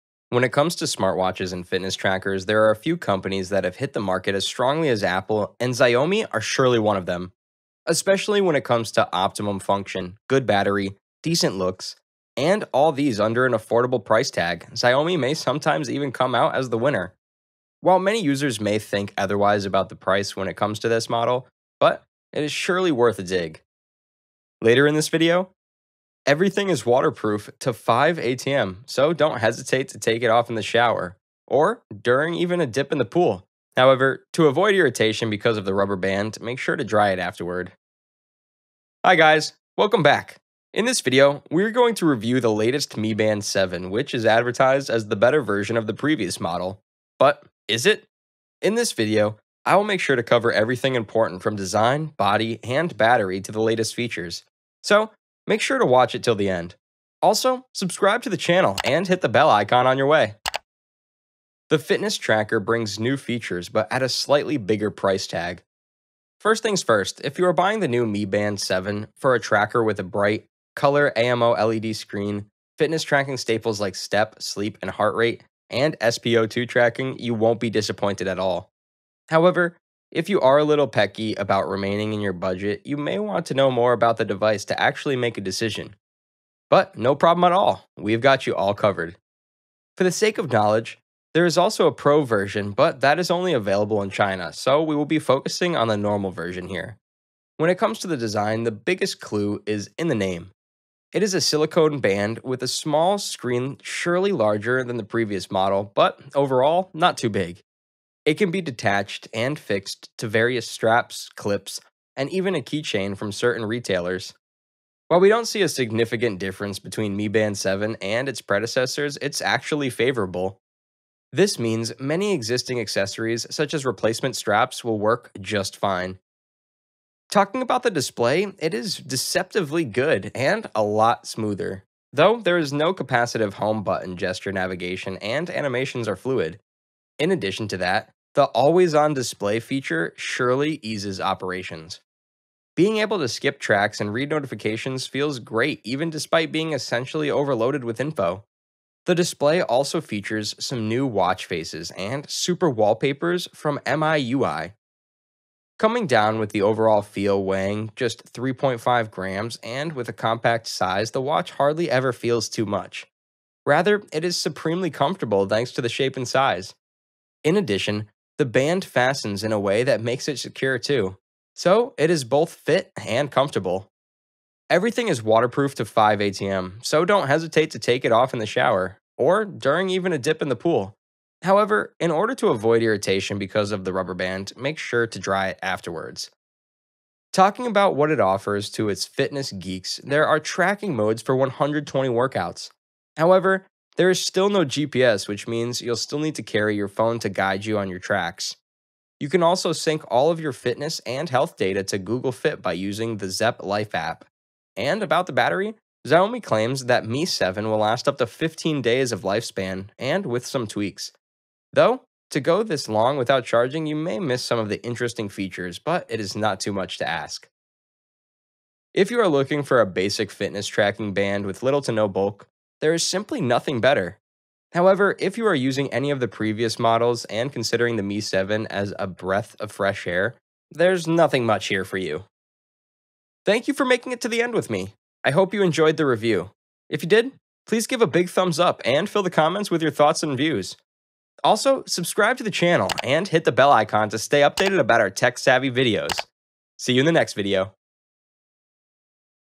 In-depth Review of Xiaomi Mi Band 7: A Truly Breathtaking Smartwatch🔥
In-depth Review of Xiaomi Mi Band 7: A Truly Breathtaking Smartwatch, In This Video We Break Down The Xiaomi band 7's Features. Watch The Video To Get All The Details.

What do you guys think about the Mi band 7 Smartwatch?